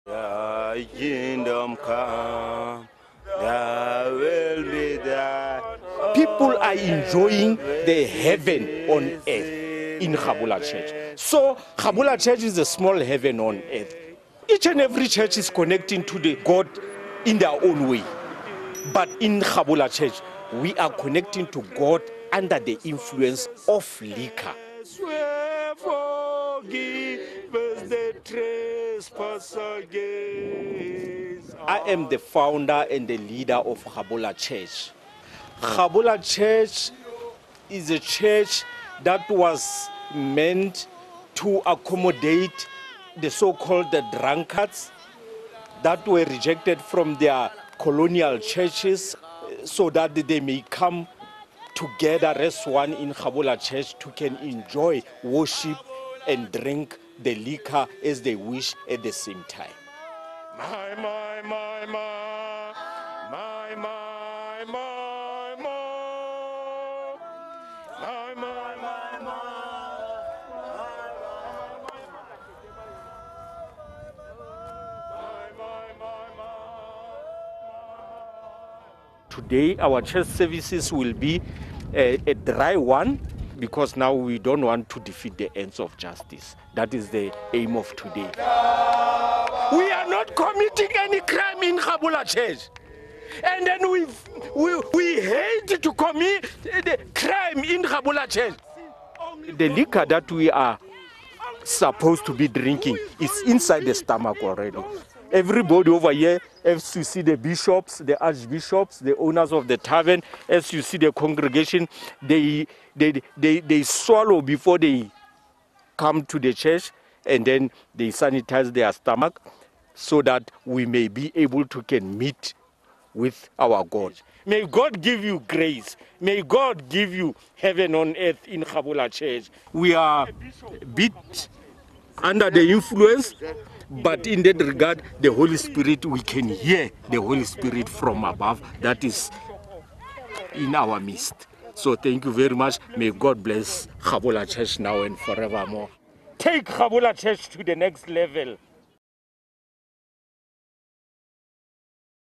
A igreja que 'conecta fiéis a Deus' com ajuda do álcool
Há muitas igrejas diferentes ao redor do mundo, mas  poucas são como a igreja Gabola, na África do Sul. "Gabola" significa "beber" na língua tswana, também falada na África do Sul. Os fiéis da igreja Gabola acreditam que sua congregação se conecta com Deus pelo consumo de grandes quantidades de álcool durante os cultos.